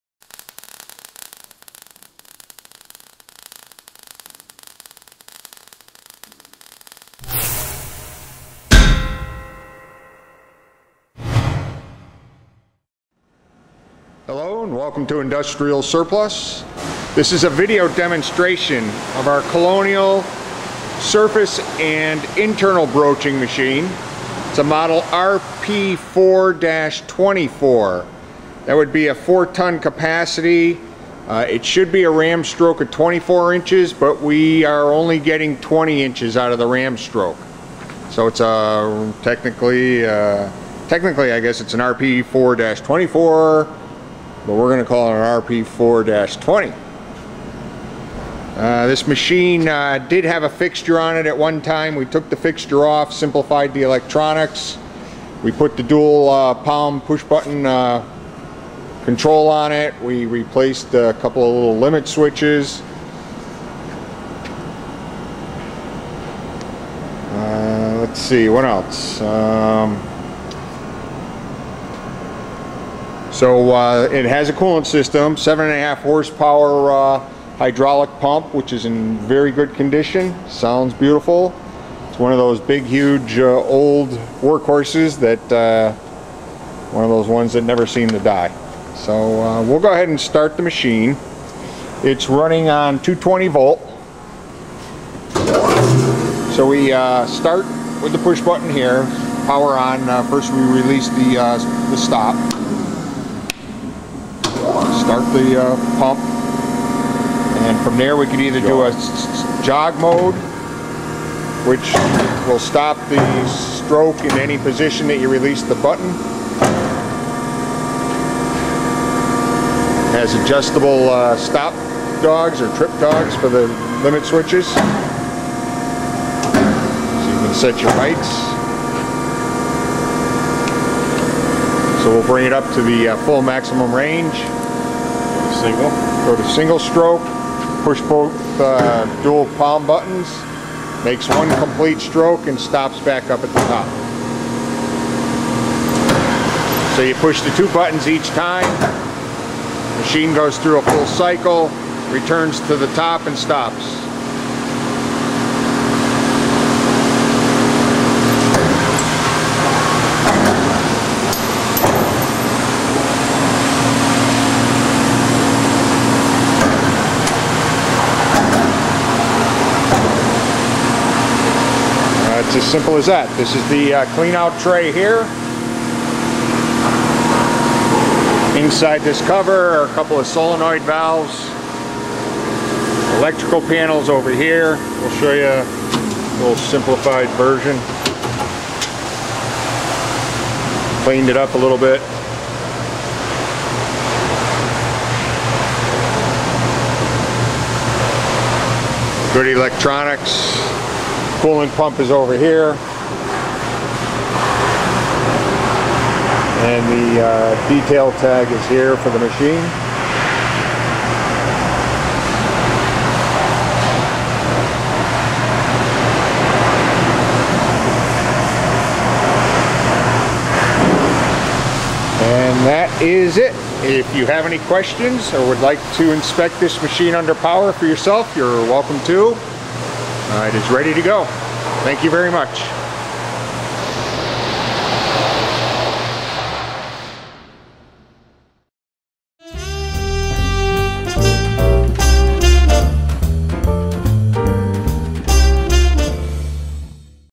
Colonial Vertical Surface & Internal Broach, Model RP4-24, Ref.#11-90 (SOLD)
Colonial Surface & Internal Broaching Machine    Ref.#11-90
Model No. RP4-24
Serial No. 54-F42-0241     New  1954

This Colonial "Ram-Press" broach allows for a variety of operations including surface broaching, internal broaching and press work operations.

Rated Capacity: 4-Tons
Ram Stroke: 20"
Overall Dimensions: 8' x 4' x 7' High

Equipped With:
Dual Palm Buttons
Inching
Coolant

7.5 HP Hydraulic Pump Motor     220/440/3/60